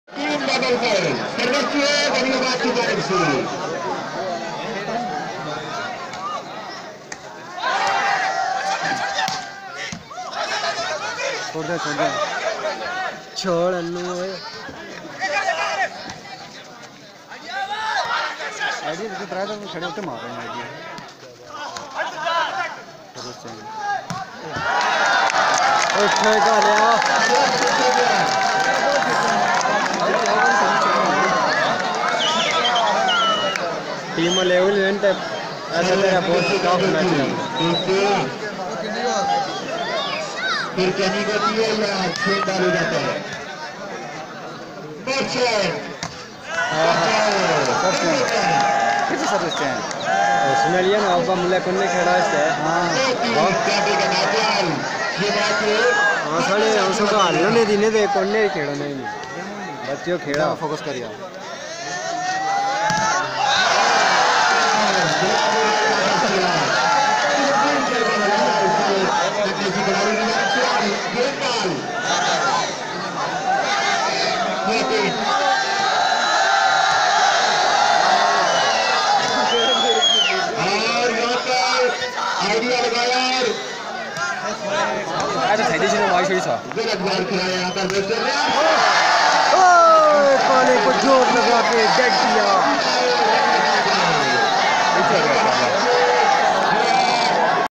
Volley ball Azad Kashmir , Rawlakot Topa vs Kuri beesbgla bagh sep 2017 pakistan
Pakistan vollyball